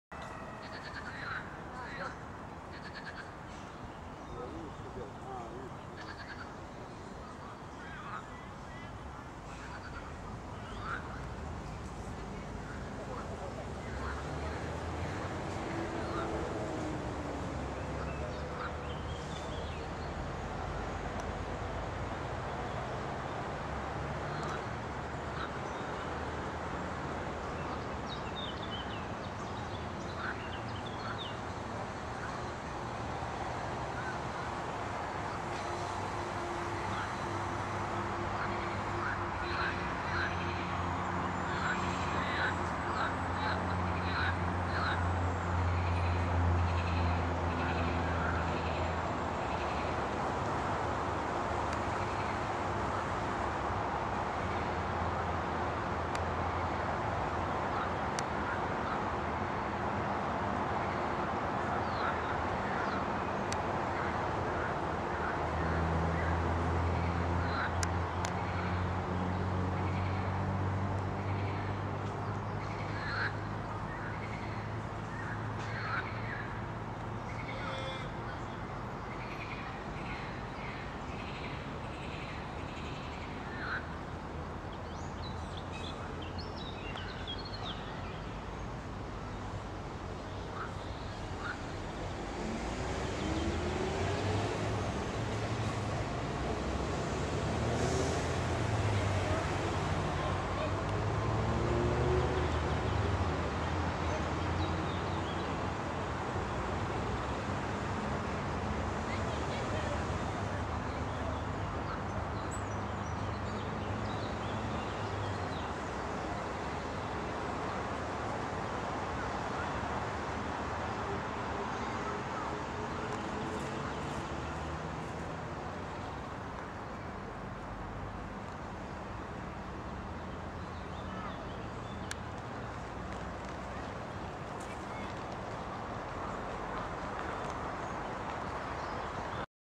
Squirrel eating bread
This squirrel, which I shot on camera in the park.